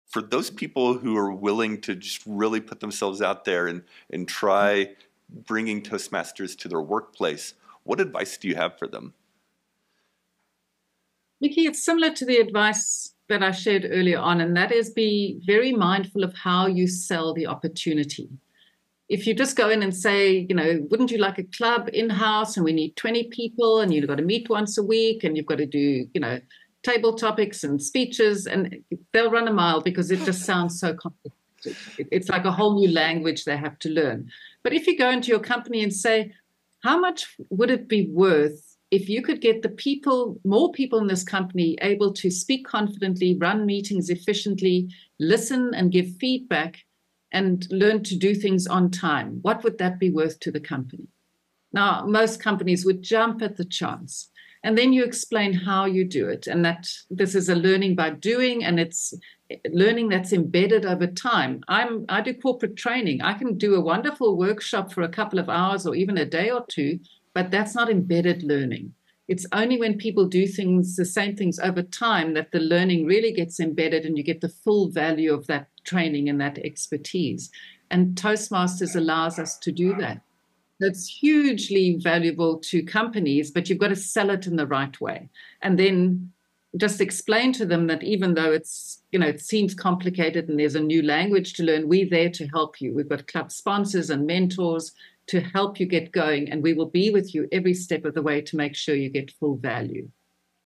Selling the VALUE of Toastmasters in the WORKPLACE
Tune into this episode of the D50AfterDarkPodcast where Aletta Rochat, DTM, reveals the effective strategies for introducing Toastmasters to the workplace. Learn how to frame Toastmasters not just as an extra activity but as a valuable investment in employee development. Aletta shares insights on how to approach companies with the benefits of Toastmasters, emphasizing improved communication skills, meeting management, timely feedback, and overall efficiency that can transform an organization’s dynamics.